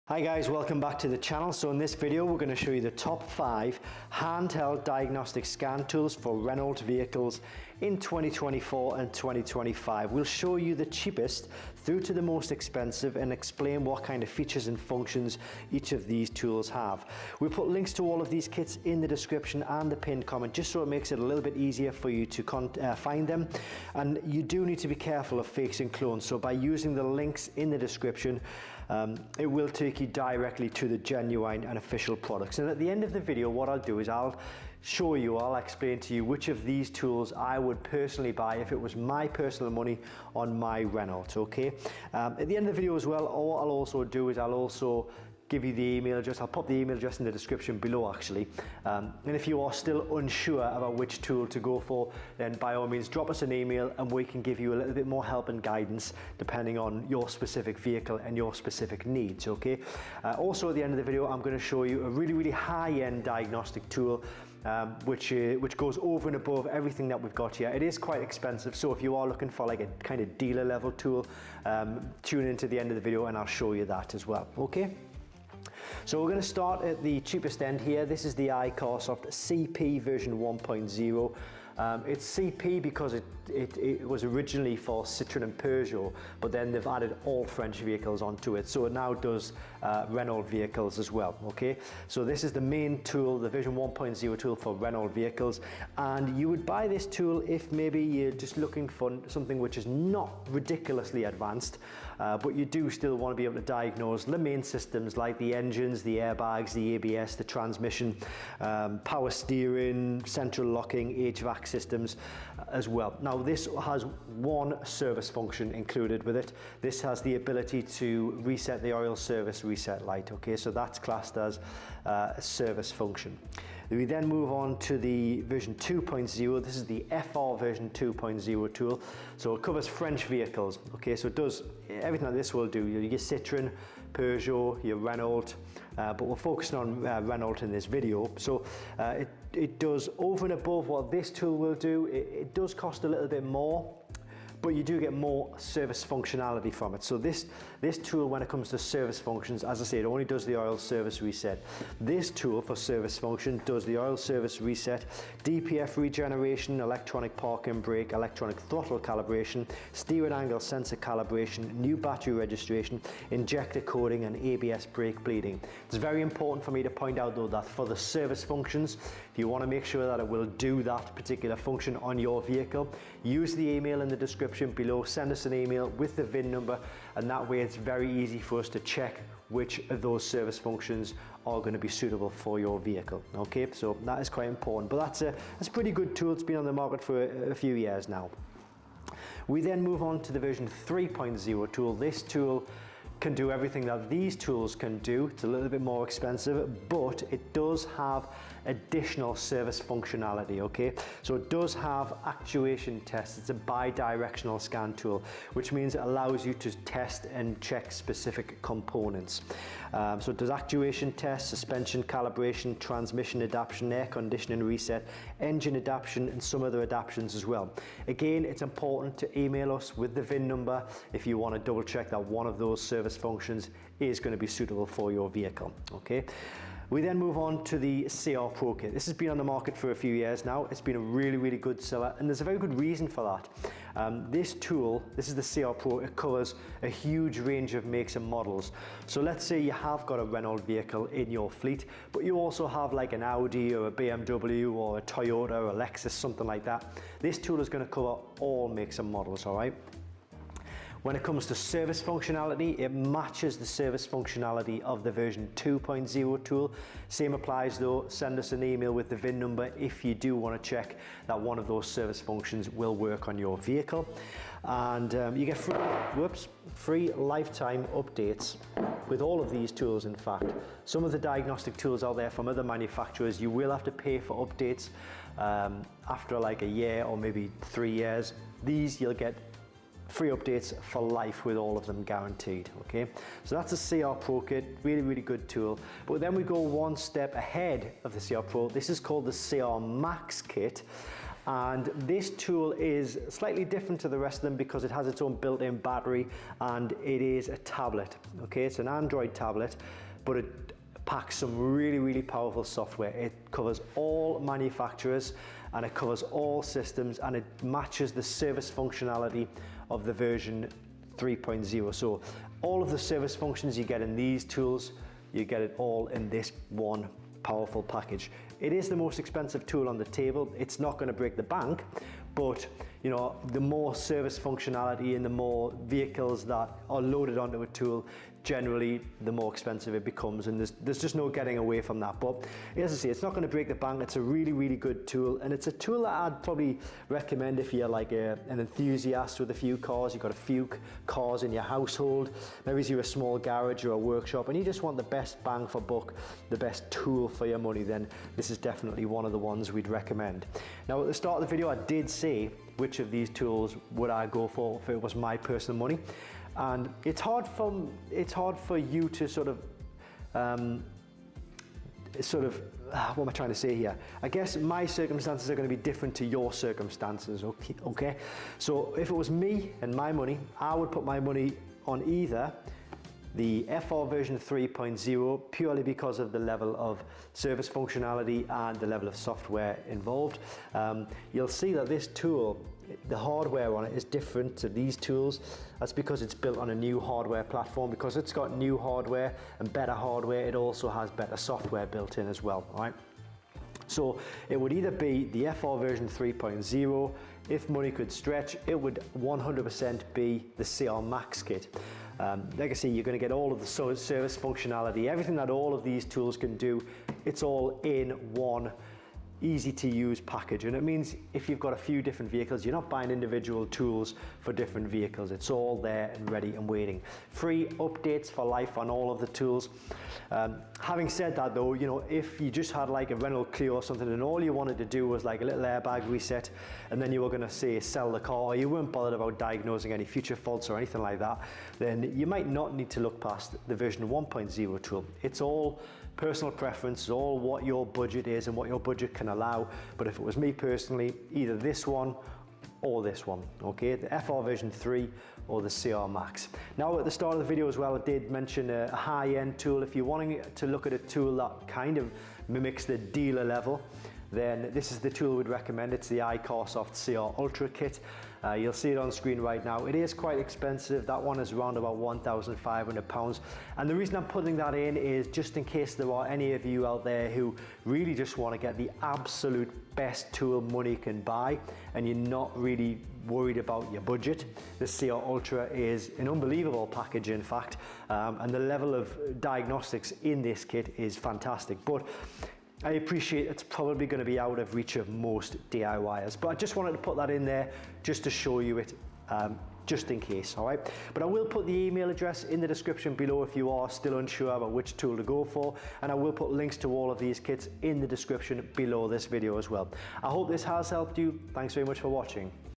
These are The Best RENAULT OBD2 Diagnostic Scan Tools - [2024 BUYERS GUIDE]
These are the best OBD2 Diagnostic Scan Tools for Renault in 2024 & 2025. These are the latest models, and will allow you to diagnose, reset warning lights like Check engine, EML, ABS, Traction, Airbag SRS, transmission, service maintenance/interval message on the dashboard of your Renault

The tools are named: iCarsoft CP V1.0, iCarsoft FR V2.0, iCarsoft FR V3.0, iCarsoft CR PRO, iCarsoft CR MAX

The tools will give you a unique fault code, and a description of the fault. You can also view Live data from all systems (depending on the tool - for example DPF live data & soot content)

The Renault vehicles that the tools can generally be used on are as follows: Alaskan
Avantime
Captur
Clio 2, 3, 4 inc V6
Espace
Kadjar
Kangoo
Kaptur
Koleos
Kwid
Laguna
Latitude
Lodgy
Master
Megane
Megane/Scenic
Modus
SM3
Scenic
SM7
Talisman
Thalia
Traffic
Twingo
Twizzy
Vel Satis
Wind
Zoe

The tools are generally manufacturerd by iCarsoft, but in some cases Foxwell & X-Tool are also considered.